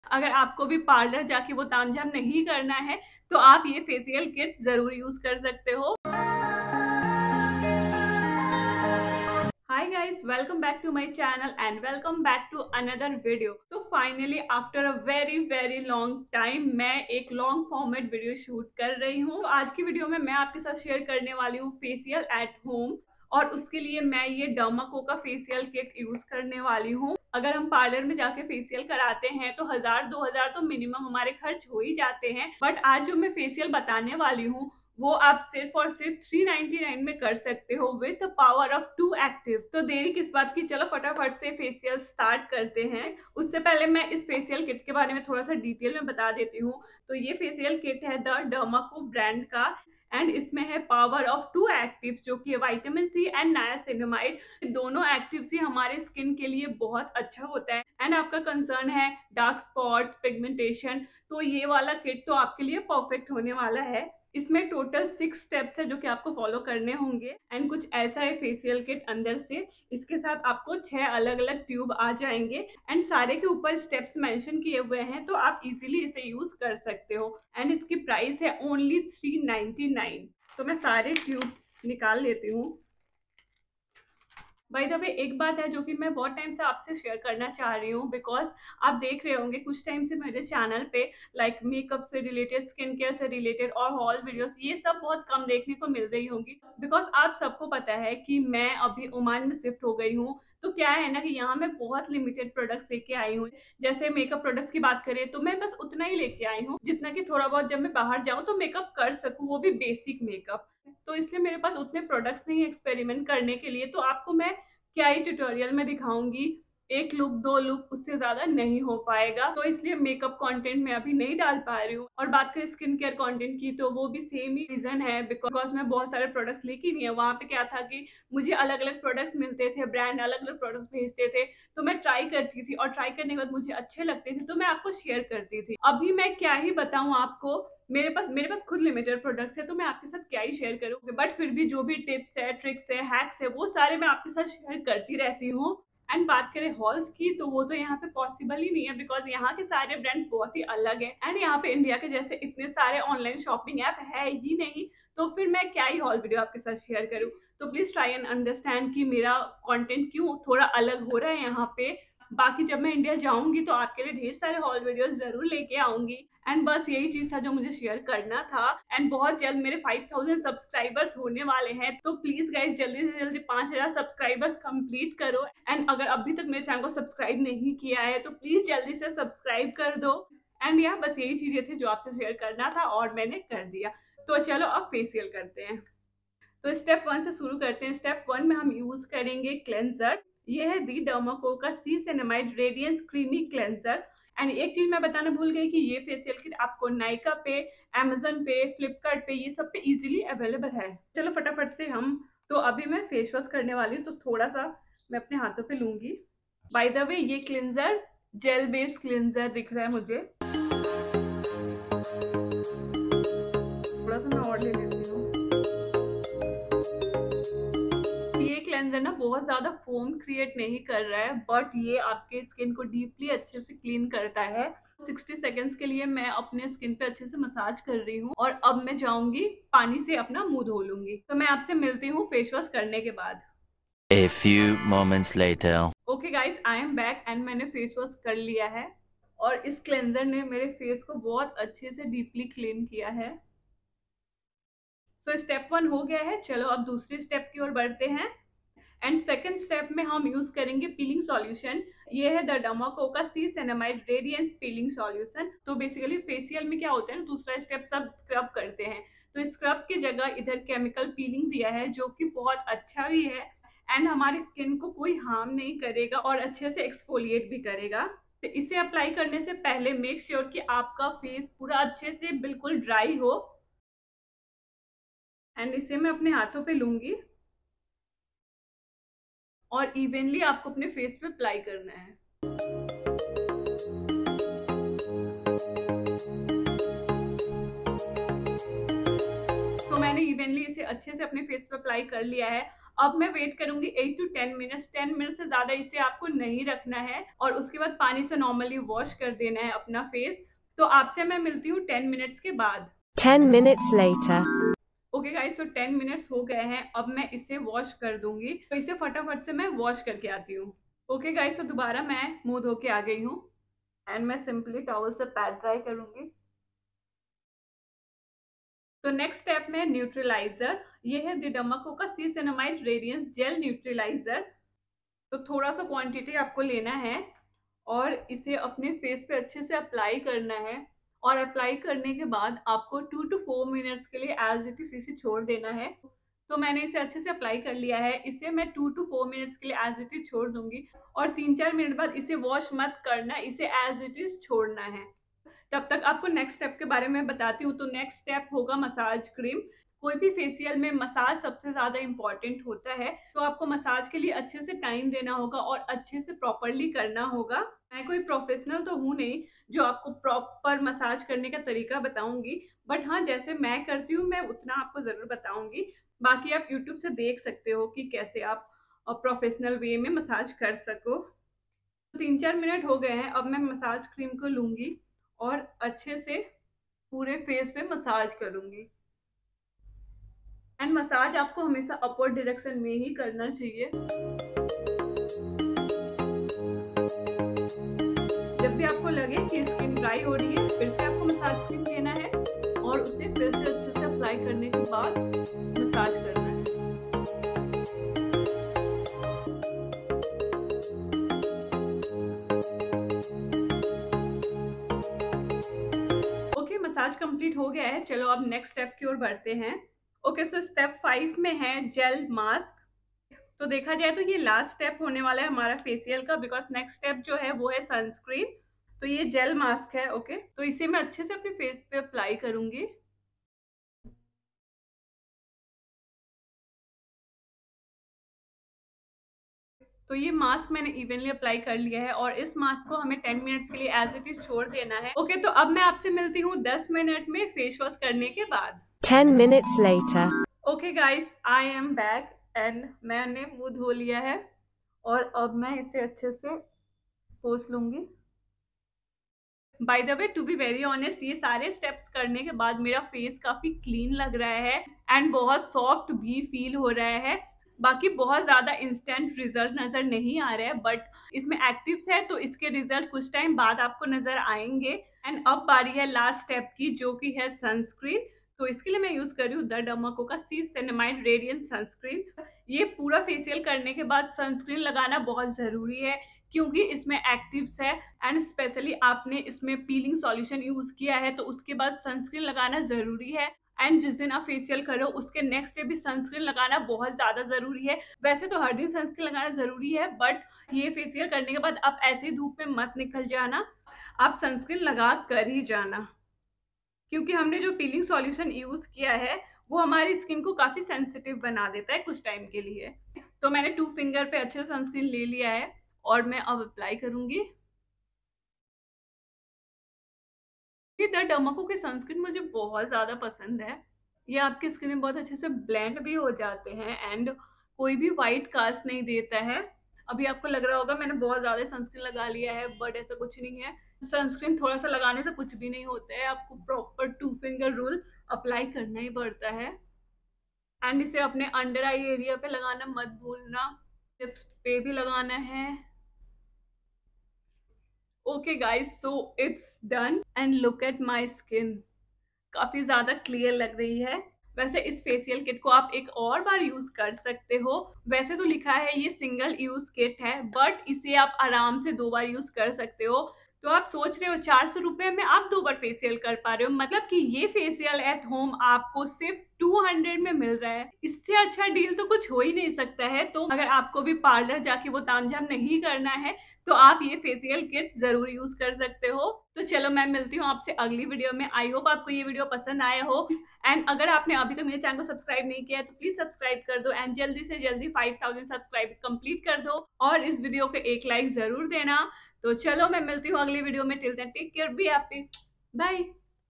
Life Update🙄+ Facial at Home🤩-The Derma Co Facial Kit👍| The Glamour World
Hey my Glamour fam! In this video I am sharing facial at home and my life update. __________________________________________________________________________________________________________________________________________________________________________